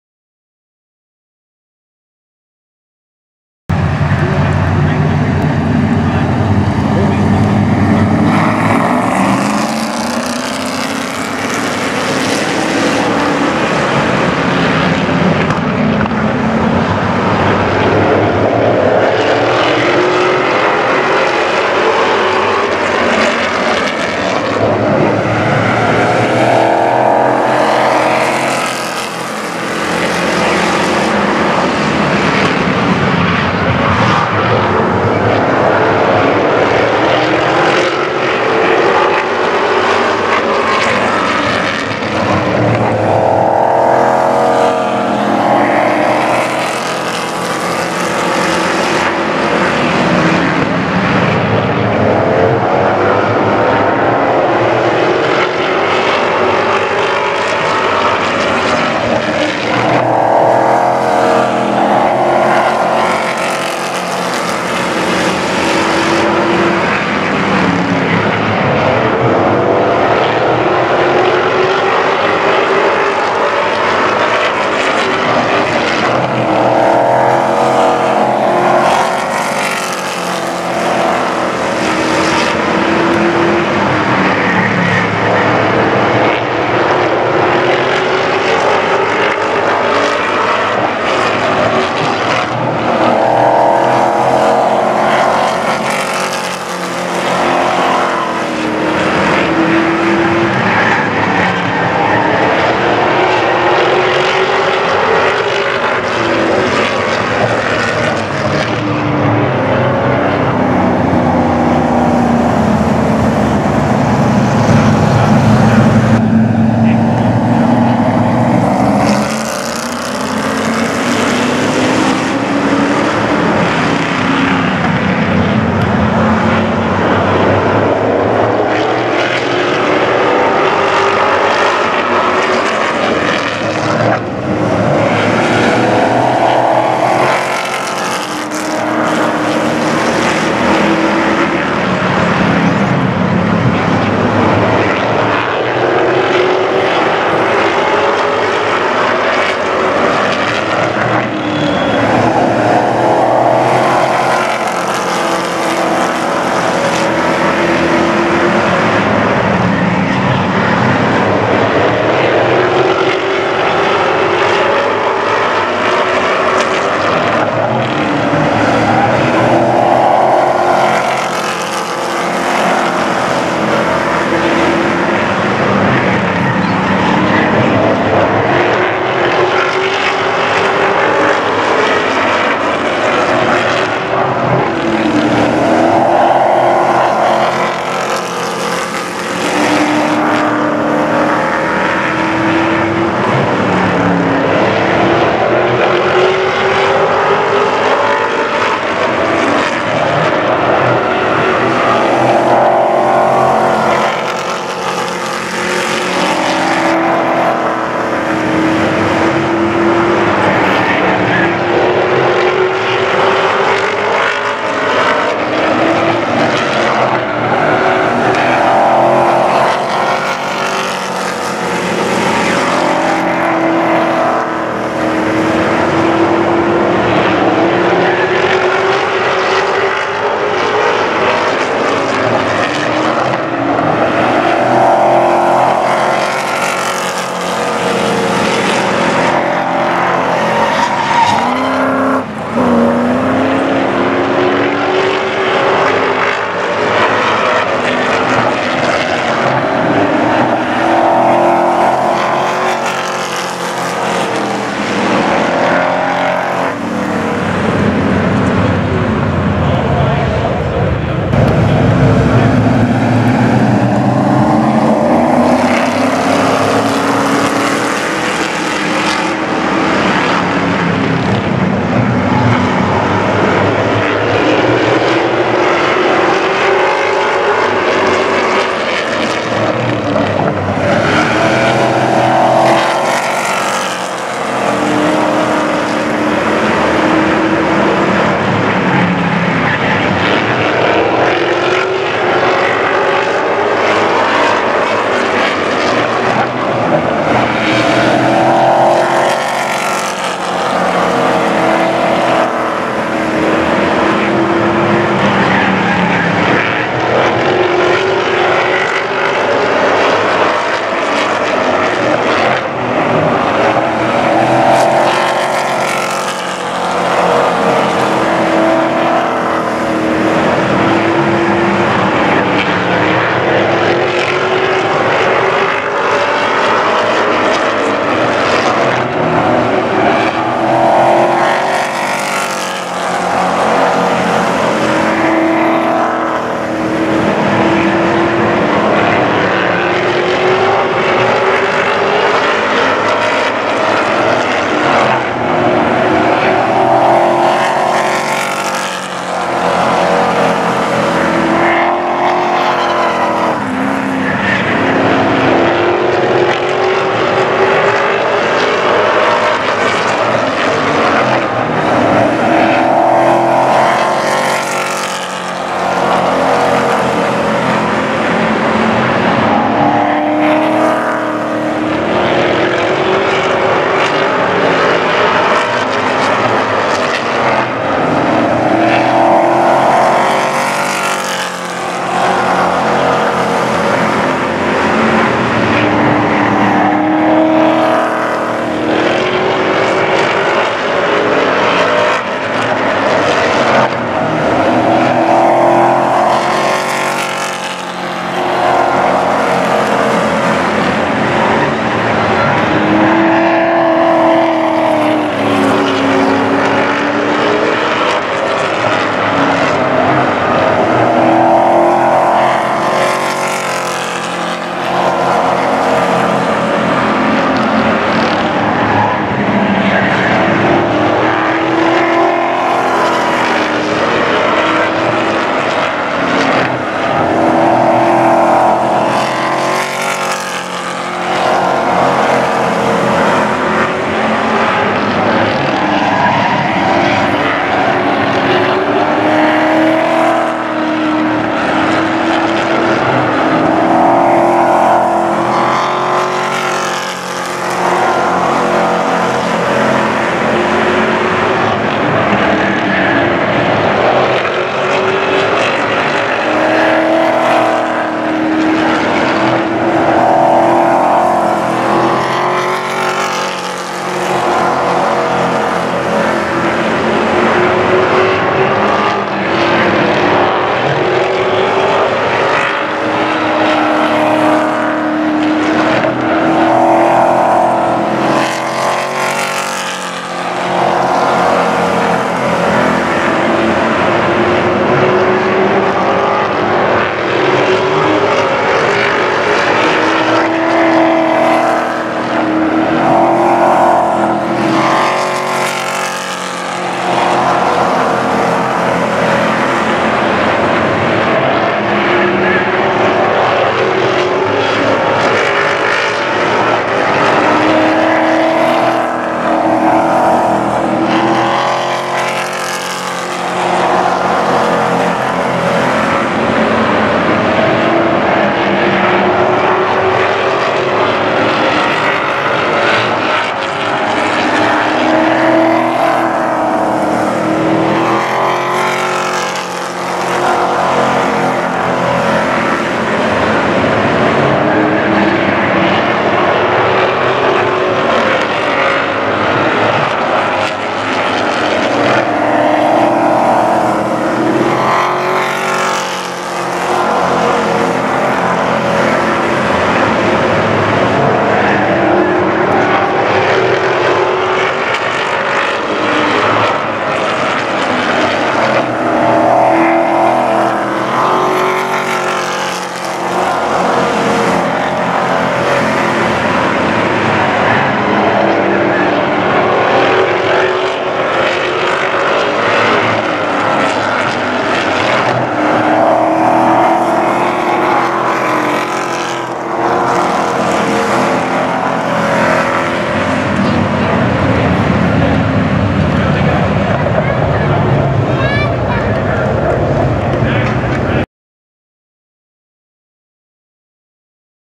Pro Trucks Feature
Fanside footage of the Pro Truck feature race.

Recorded August 15th/2020 @Wyant Group Raceway in Saskatoon.